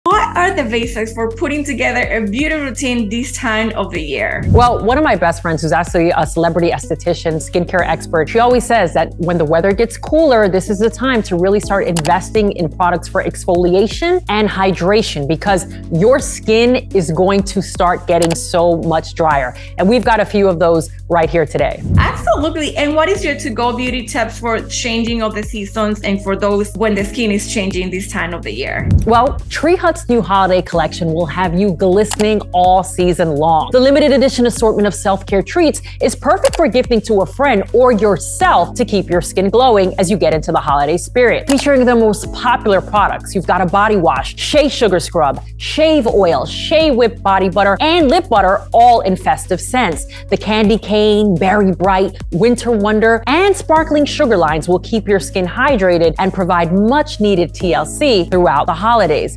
These are the basics of having a beauty routine at this time of year
Mili almovodar is going to be telling us what are the bases to have a beauty routine at this time of the year
As seasons change, the combination of rainy days and cool and hot weather can cause dry hair and skin. The changing temperatures make it essential to change your beauty routine, too. People realize their skin and hair are drier during winter but may not know that their entire body needs special attention this time of year. That’s why we have the perfect celebrity beauty expert to help.

Milly Almodovar is a well-known and popular New York-based beauty expert and TV lifestyle host. You’ve seen her on Today, Wendy Williams, Steve Harvey, and many other shows. Discover how Milly turned her love of fashion into a fantastic career as she shares her super secrets.

MILLY’S MARVELOUS SEASONAL BEAUTY SECRETS INCLUDE:

DRESS TO THE 9’S: Learn which accessories and cosmetics are trending on New York and Paris runways

ABOUT FACE: What you eat and drink ‘reflect’ in your complexion

SKIN TO WIN: How to look and feel good

FRESHEN UP: Seasonal makeup options